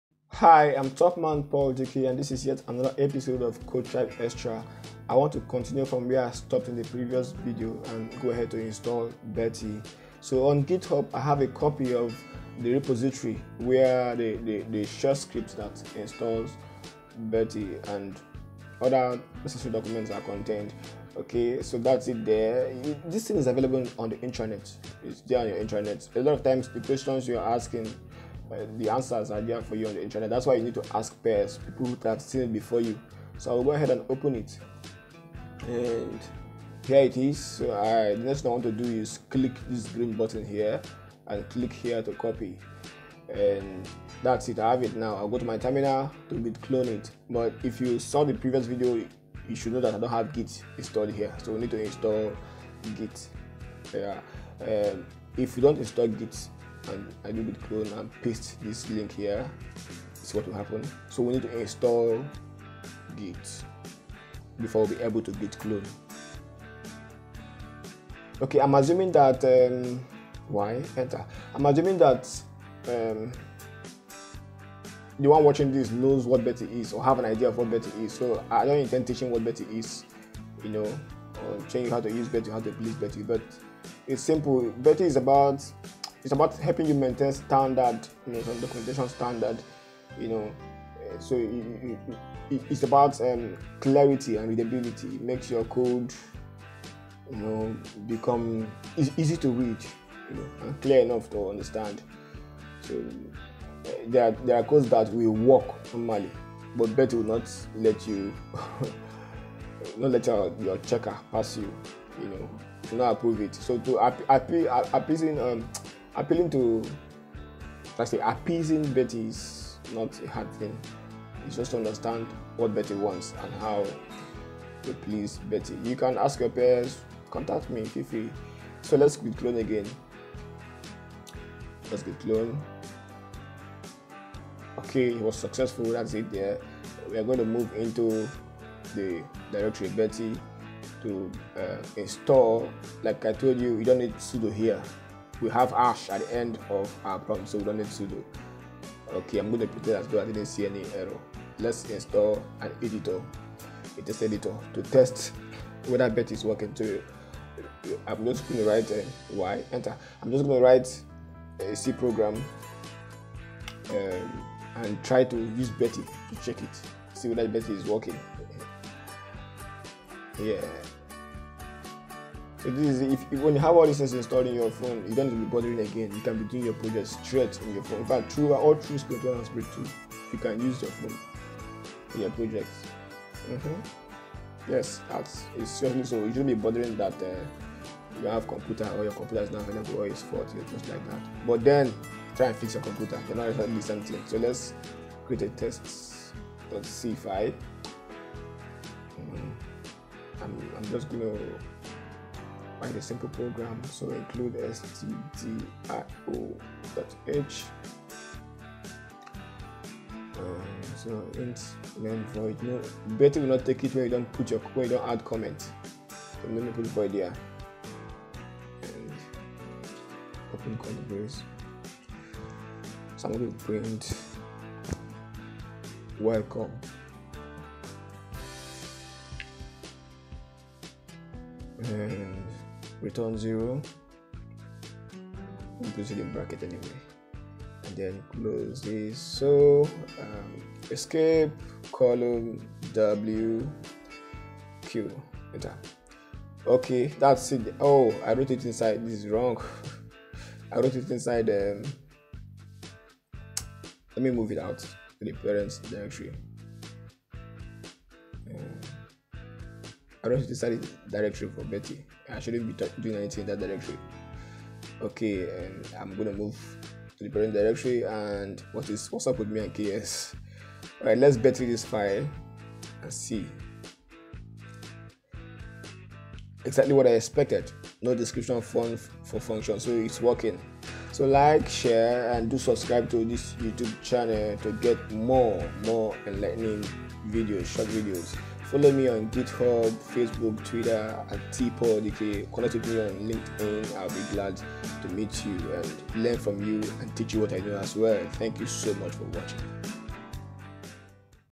How to Install Betty on your phone
A brief demo on the processes involved in having betty installed and how to use it.
It was done on an android phone but it's the same procedure for any linux system.